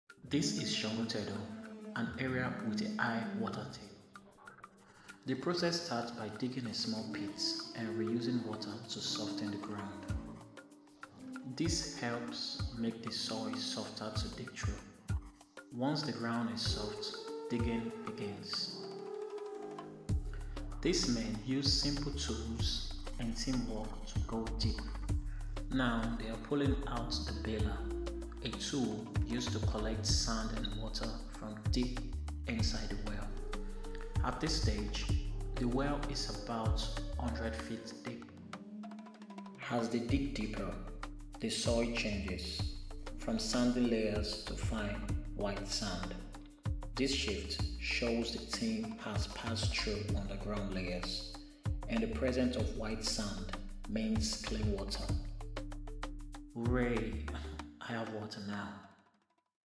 Manual Borehole Digging in Sangotedo – 100ft Deep!
Watch how a deep water well is manually dug in Sangotedo, Lekki — starting with a recycled water pit and ending with clean water at over 100-feet deep. No machines. Just skill, teamwork, and grit.

This shows the traditional method of digging a borehole in areas with a high water table.

📍 Location: Sangotedo, Lekki, Lagos
💧 Water depth: approx. 100-feet
📦 Soil types: sandy layers, fine white sand